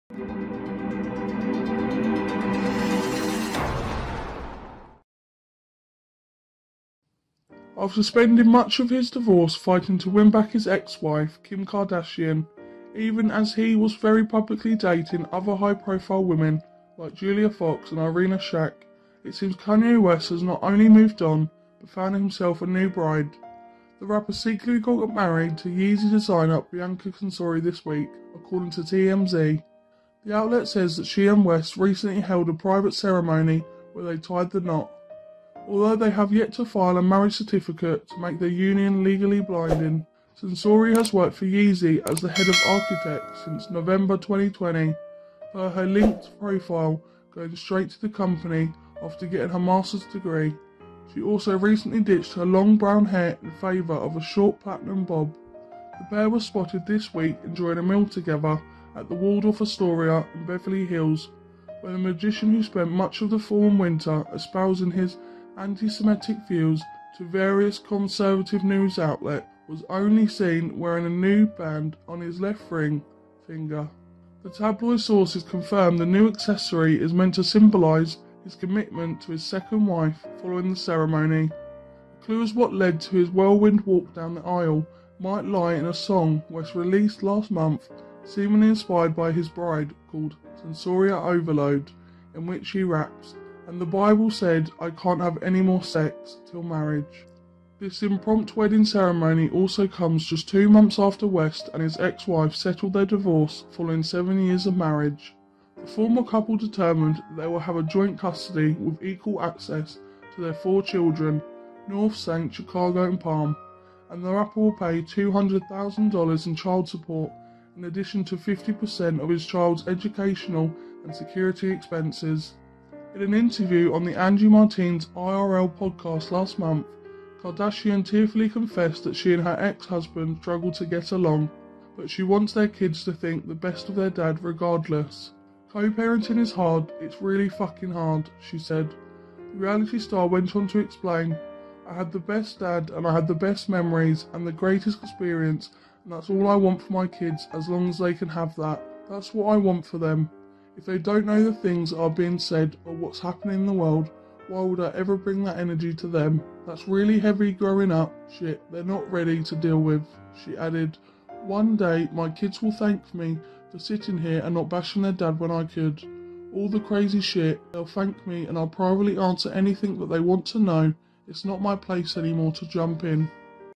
CONFIRM: Kanye West Gets Secretly MARRIED To Bianca Censori
Kanye West is an American rapper, singer, songwriter, record producer, and fashion designer. Although Kanye West and Bianca Censori have been seen wearing wedding rings, at the moment there is no official document confirming the alleged wedding between the two, but the Australian architect's relatives seem to confirm the fact thanks to their recent comments on social media.